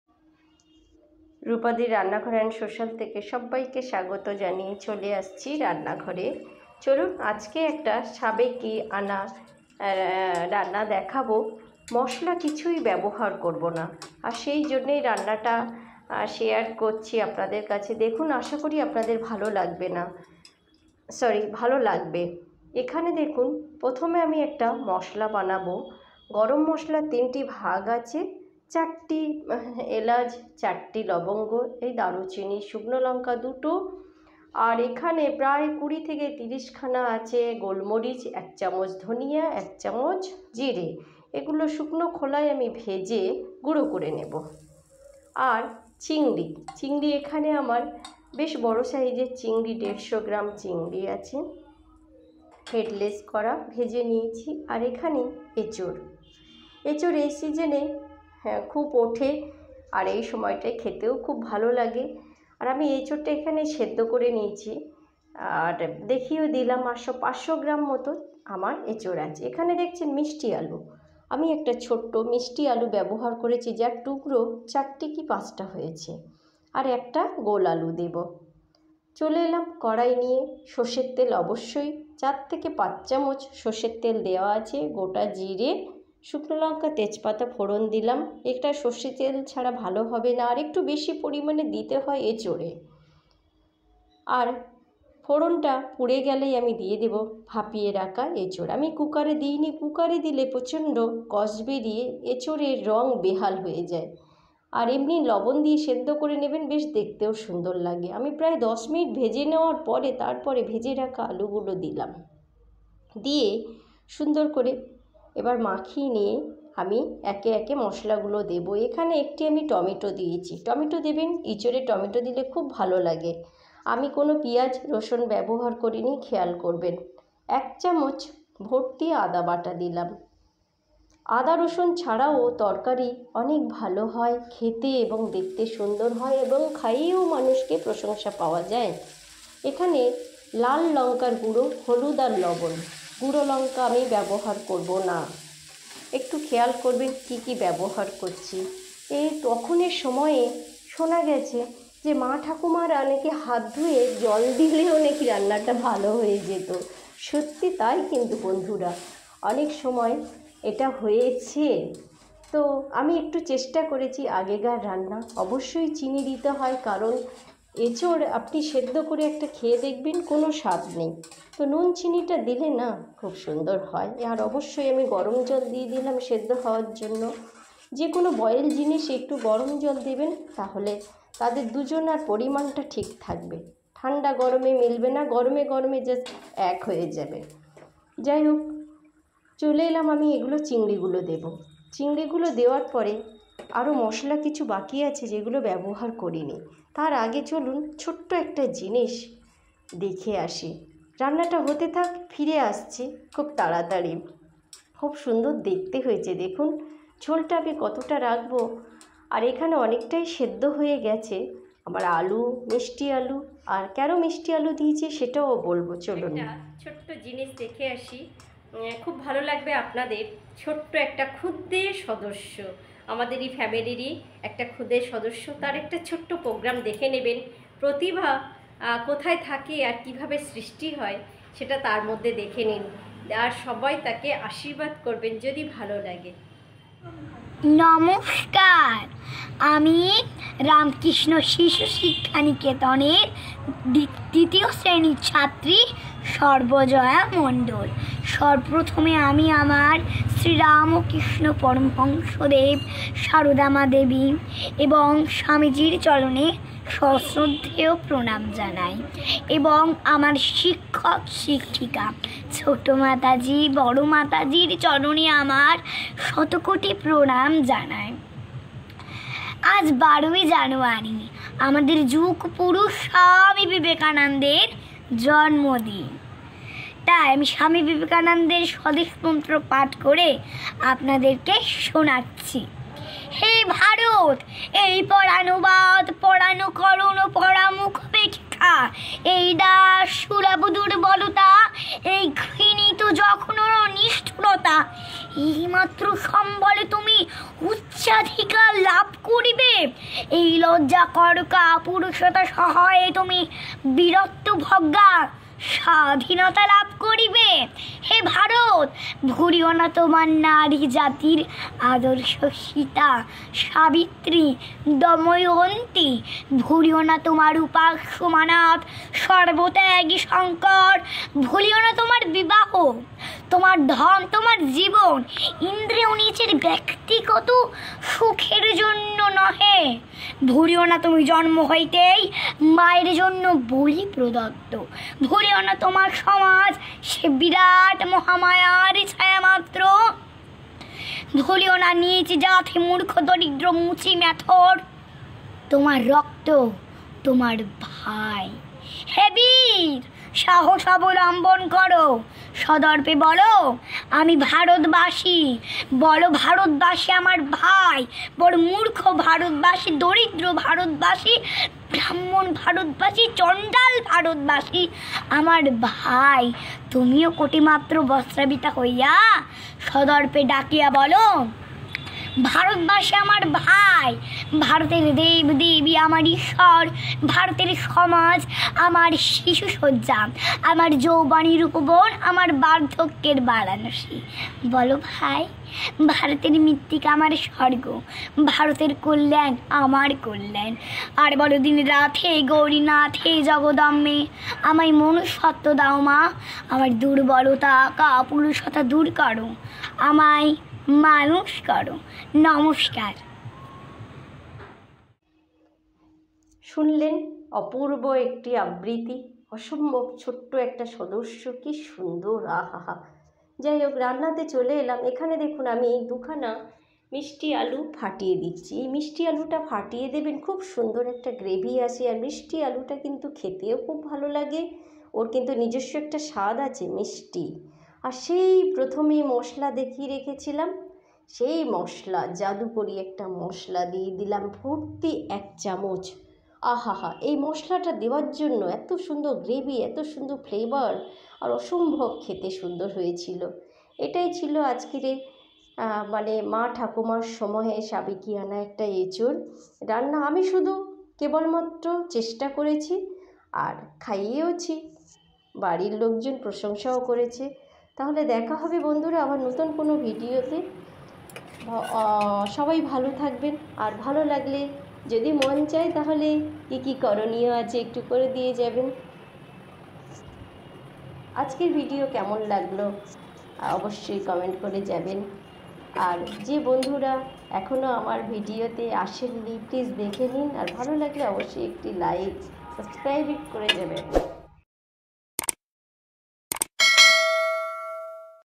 এঁচোড়ের তরকারি।পিয়াজ রসুন ছাড়া না দেখলেই ভাবতে পারবেন না। Echor Curry .
70 দশকের মায়ের ঠাকুমার হাতে একটি রান্না আদা রসুন ছাড়া স্বাদে গুণে ভরপুর। নতুন কিছু আছে অবশ্যই ভিডিওটা দেখুন এবং ভালো লাগলে অবশ্যই আপনারা বানান খুবই সুন্দর একটি রেসিপি।